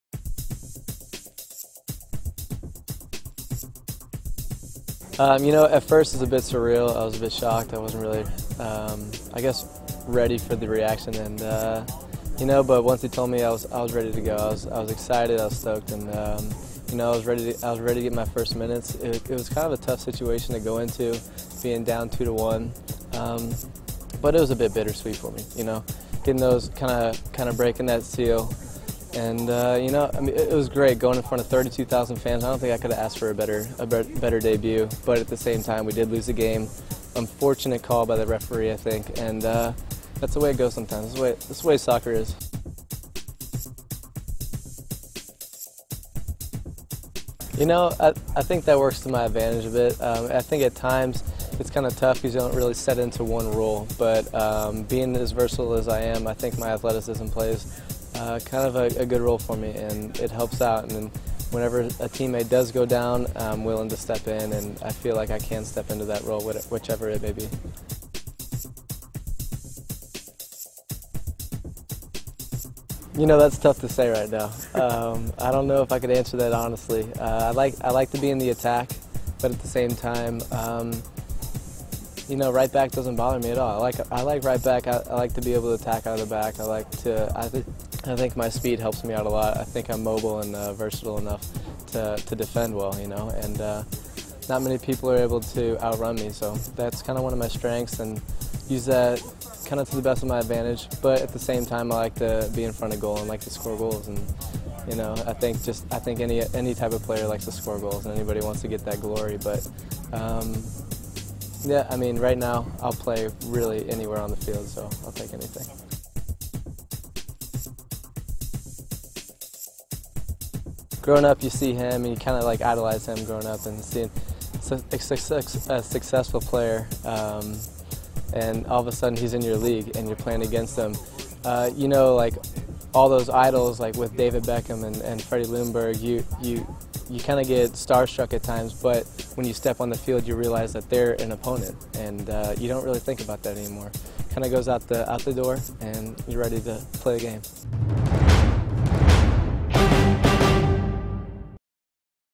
Erik Ustruck Interview About MLS Debut
Erik Ustruck Talks about his MLS Debut